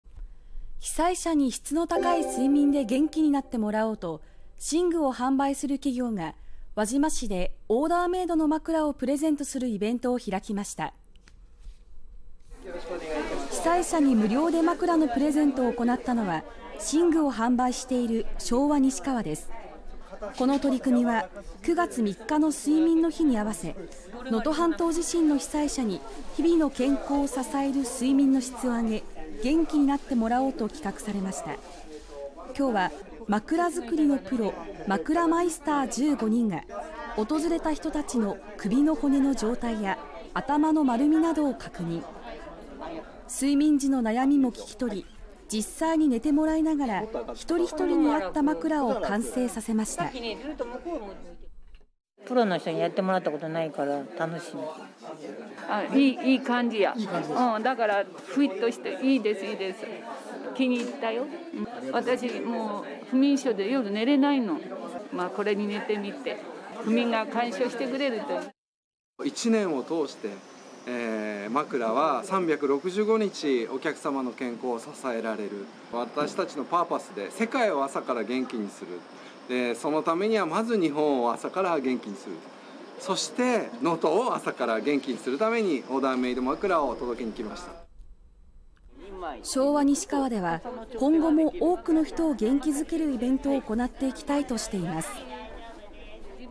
被災者へ 睡眠の日にオーダーメイド枕をプレゼント
被災者に質の高い睡眠で元気になってもらおうと
寝具を販売する企業が輪島市で
オーダーメイドの枕をプレゼントするイベントを開きました。
被災者に無料で枕のプレゼントを行ったのは
寝具を販売している昭和西川です。
この取り組みは９月３日の睡眠の日に合わせ、
能登半島地震の被災者に日々の健康を支える睡眠の質を上げ
元気になってもらおうと企画されました。
９月３日は枕づくりのプロ「枕マイスター」１５人が
訪れた人たちの首の骨の状態や頭の丸みなどを確認。
睡眠時の悩みも聞き取り、
実際に寝てもらいながら一人一人に合った枕を完成させました。
昭和西川では今後も多くの人を元気づけるイベントを
行っていきたいとしています。